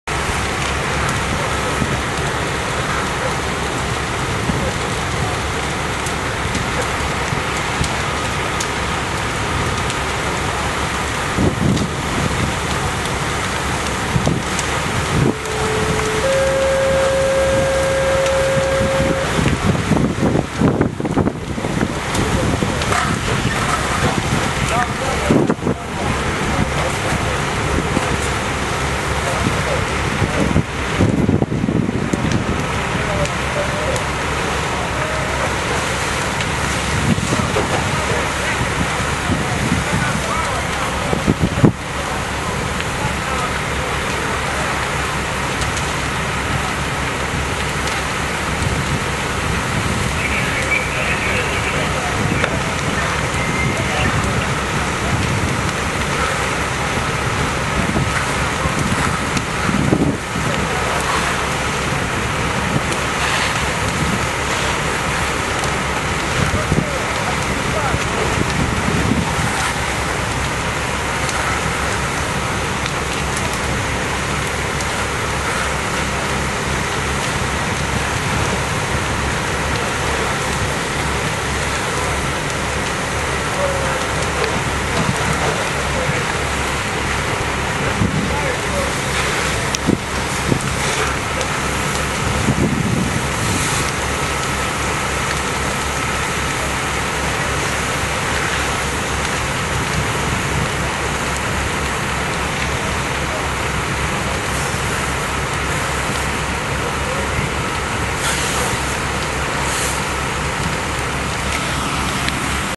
Harvey, Illinois Box Alarm
Just a short video to show the wind conditions faced by the Harvey Fire
Department on October 24, 2015. Most efforts were spent successfully
protecting occupied exposures. Clearly I am not a professional
videographer. I know the white balance is incorrect and that the video
shakes from time to time. I realize that there are some who will want
to point out the obvious, however.